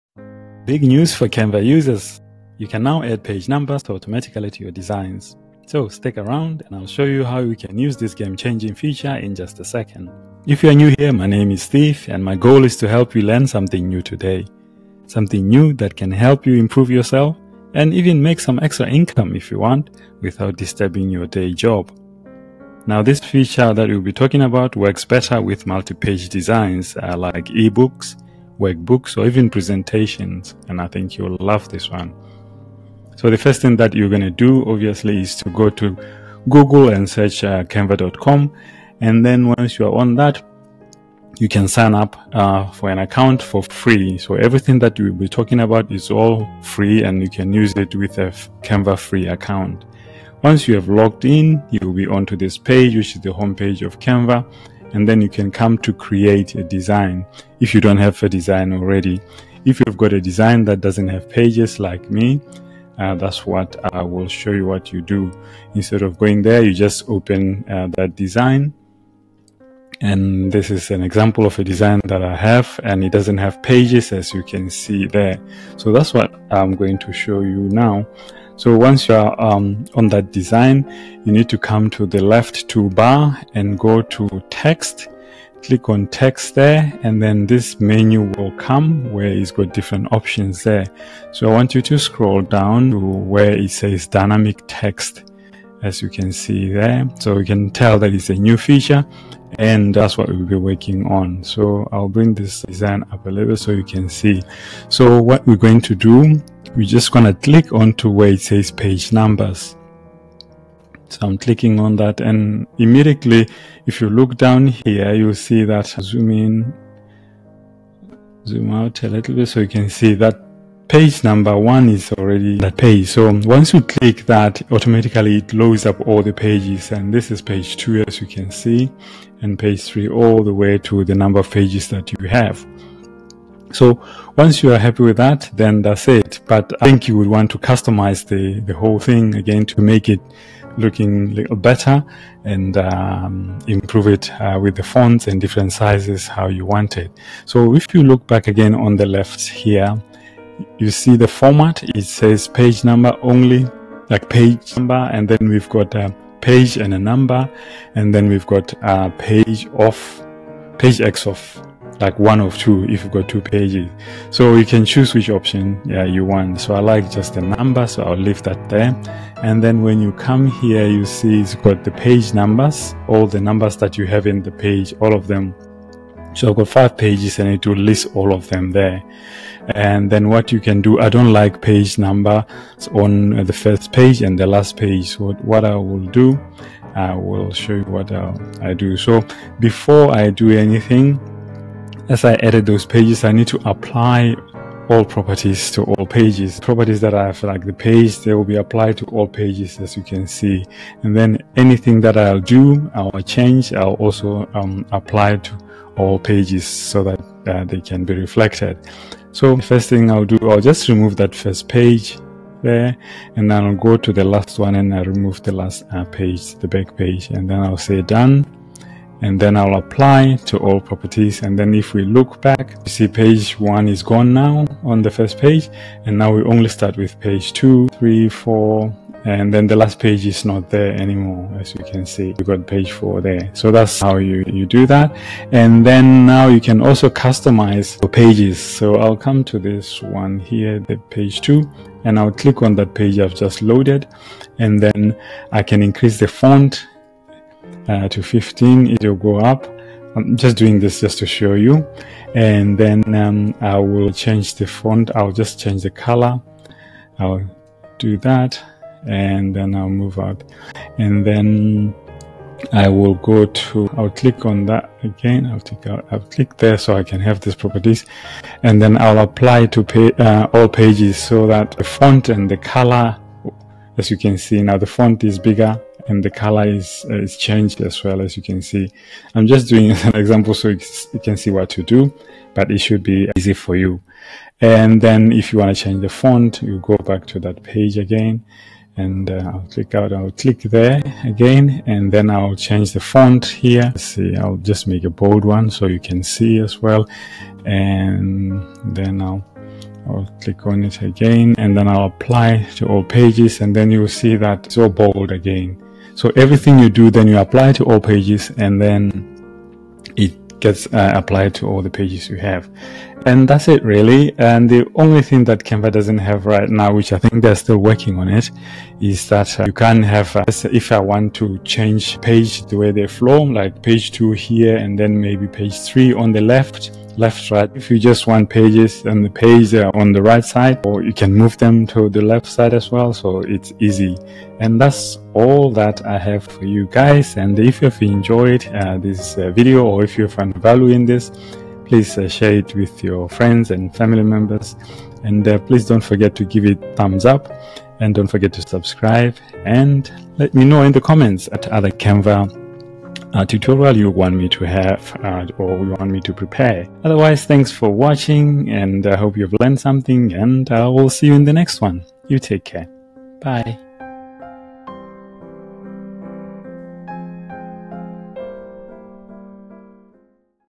How To ADD Page Numbers In A CANVA Design Like A PRO
Are you looking for an easier way to add page numbers to your Canva designs? In this video, I’ll show you how to use Canva’s latest Page Numbering Feature, a game-changer for creating multi-page documents like eBooks, workbooks, and presentations. Whether you’re a designer, teacher, or content creator, this step-by-step tutorial will help you save time and ensure your projects look polished and professional.

No more manual page numbering—just a few clicks, and you’re done! I’ll also cover how to customise the page numbers to match your design style and export your project seamlessly.

👉🏻 What You'll Learn in This Video:

➡️ How to add page numbers in Canva.
▶️ Customising page numbers (placement, font, size, and colour).
▶️ Tips for multi-page projects like reports, eBooks, and more.

Canva’s new feature is perfect for anyone, whether you're a professional, a student, or working on multi-page designs.
Give it a try today!

** Unbreakable: The Power of Resilience In Overcoming Challenging Times

00:00 Introduction
00:34 SignUp For a Free Canva Account
00:52 How to add page numbers in Canva
03:55 Customising page numbers (placement, font, size, and colour)
05:13 Summary
05:34 CTA